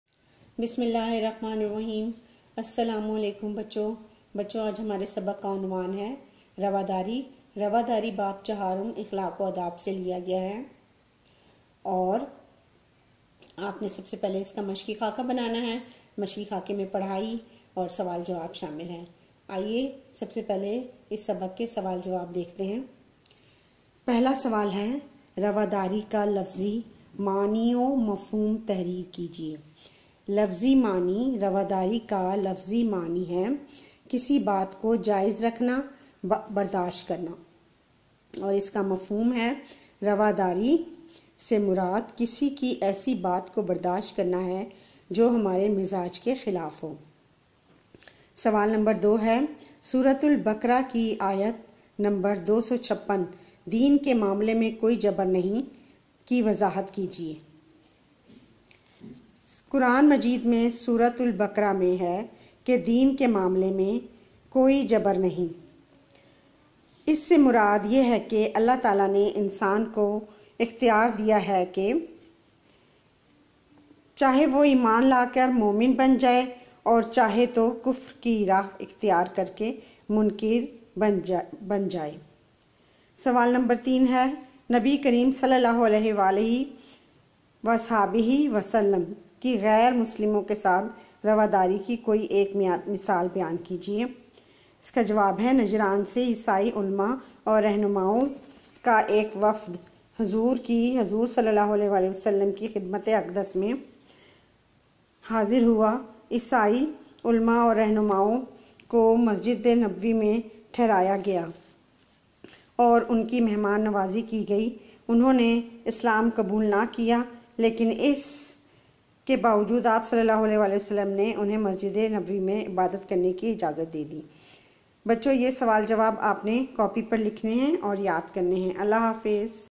جماعت پنجم ,مضمون: اسلامیات,باب چہارم اخلاق و آداب ,عنوان: رواداری
Video from The Educators Taxila

معلمہ: عفت مآب